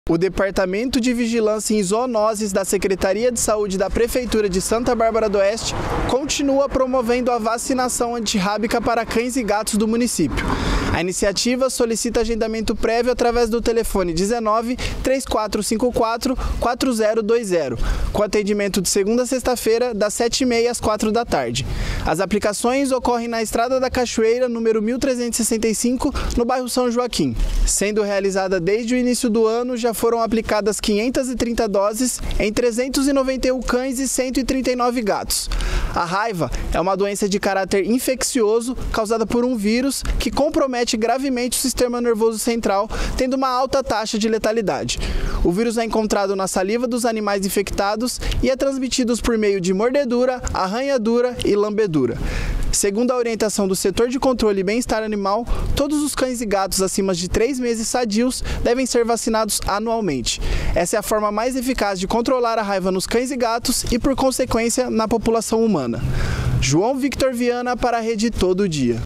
SANTA BÁRBARA: SEGUE VACINAÇÃO ANTIRRÁBICA PARA CÃES E GATOS
BOLETIM - JOÃO VICTOR VIANA